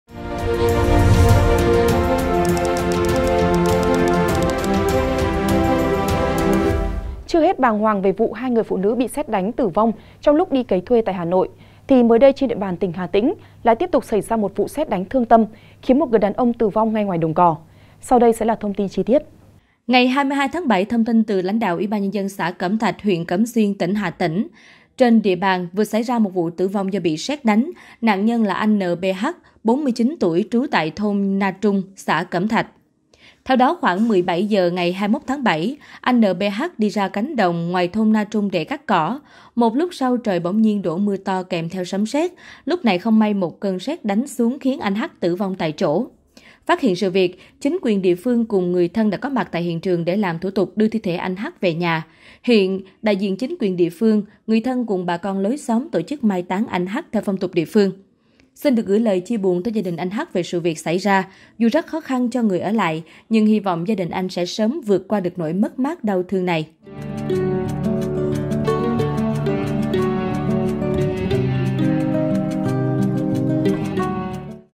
Bàng Hoàng Người Đàn Ông Bị Sét Đánh Tử Vong Tại Chỗ Khi Đang Cắt Cỏ Ngoài Đồng | SKĐS
SKĐS | Mới đây, trên địa bàn tỉnh Hà Tĩnh vừa xảy ra một vụ sét đánh thương tâm khiến một người đàn ông tử vong ngay ngoài đồng cỏ. Phát hiện sự việc, chính quyền địa phương cùng người thân đã có mặt tại hiện trường để làm thủ tục đưa nạn nhân về nhà.

Mời quý vị và các bạn xem thêm: Vụ bé V.A Tử Vong: Mẹ Ruột “Dì Ghẻ” Thanh Minh Con Gái Bị Rối Loạn Tâm Thần Hậu Covid